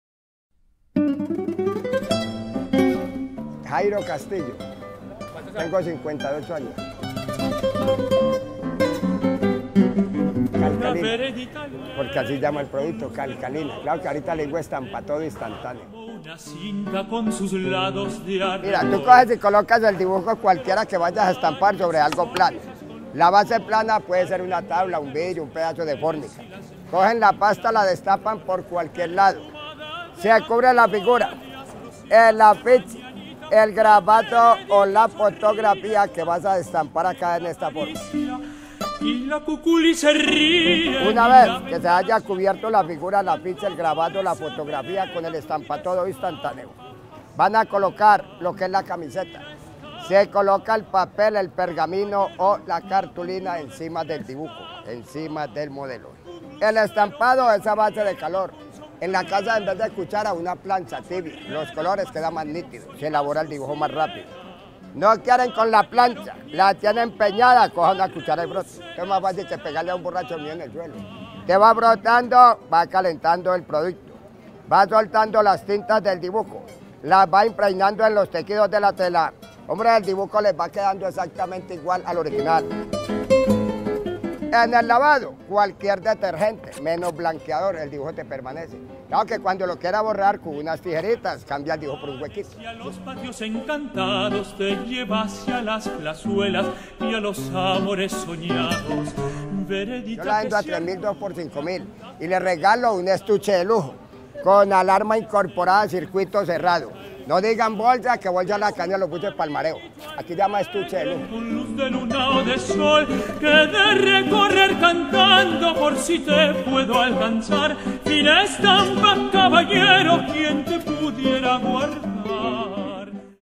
Tecnología de Calle
Nuestro equipo salió a caminar por el centro y allí se dio a la tarea de desempolvar una sección dedicada a todo eso que nos encontramos en la calle. Esta vez estuvieron conociendo el trabajo de un señor que se dedica a estampar con un jabón especial.